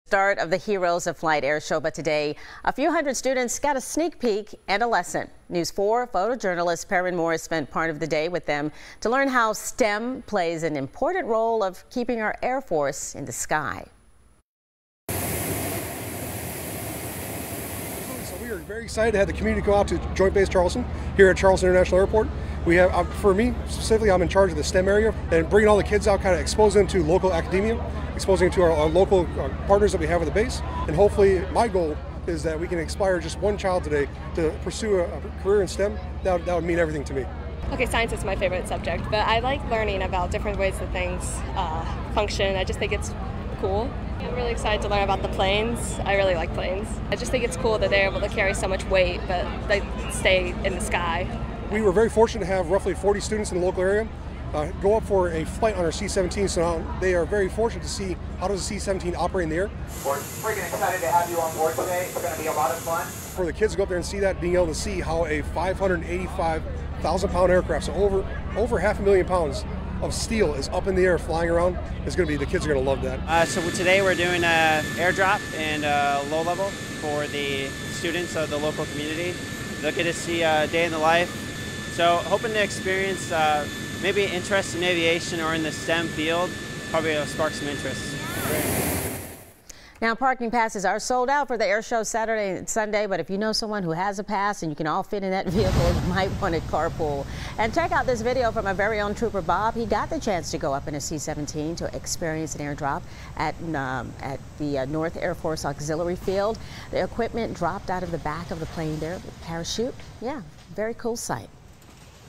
Students receive a fun lesson in flight at Charleston Airshow
NORTH CHARLESTON, S.C. (WCIV) — The "Heroes of Flight" Airshow will take place from April 19 to April 21 at Joint Base Charleston.

The event features the U.S. Air Force Thunderbirds, aerial performances, and over 40 static aircraft, according to the base.